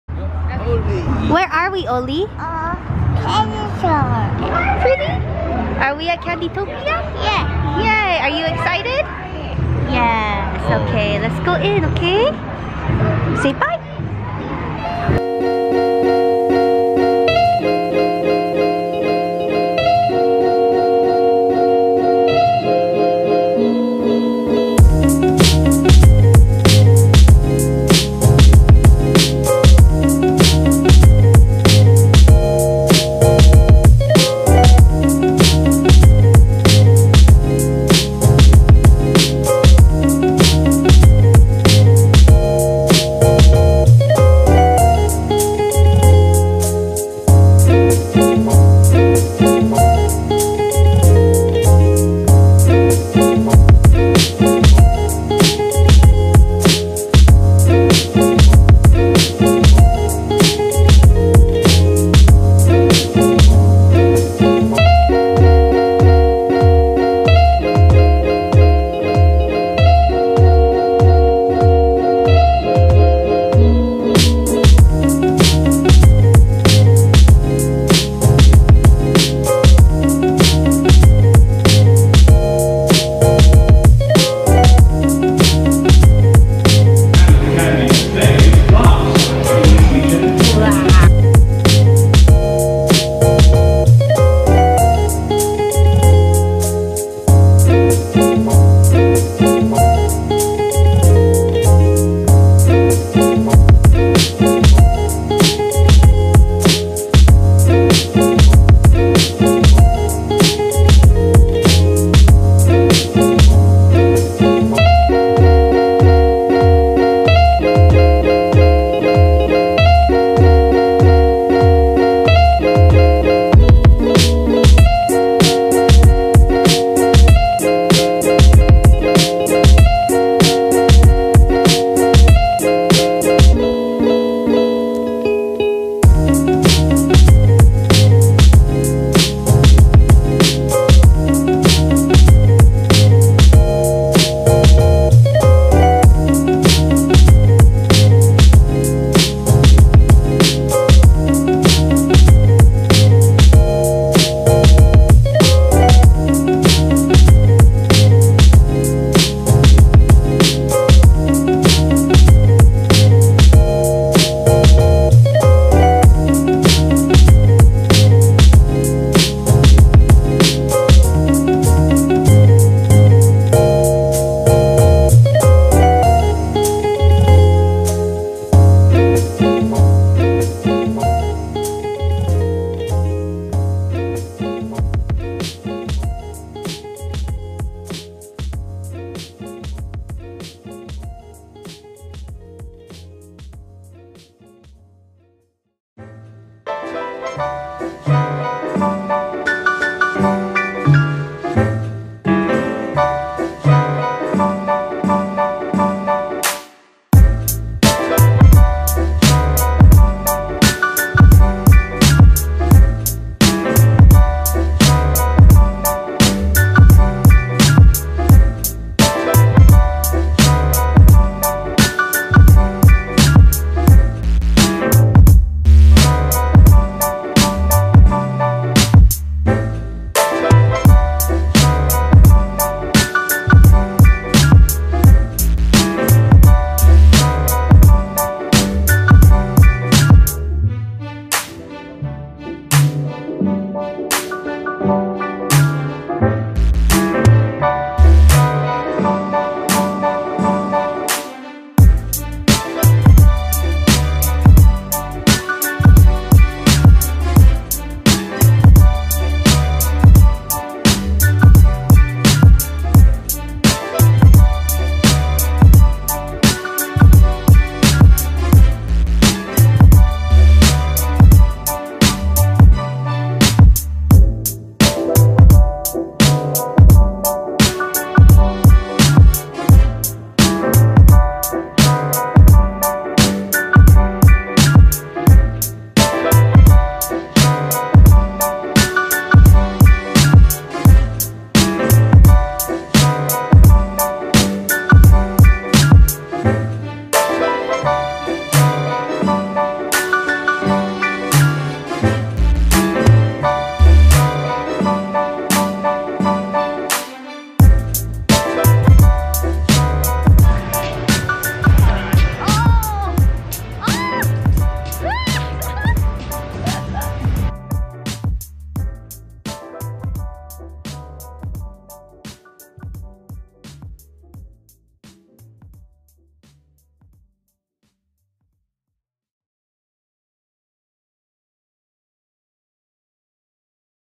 FOLLOW US TO CANDYTOPIA!
We had a blast at Candytopia in Dallas, TX!

All mail & packages send to:

3044 Old Denton Rd. STE 111-117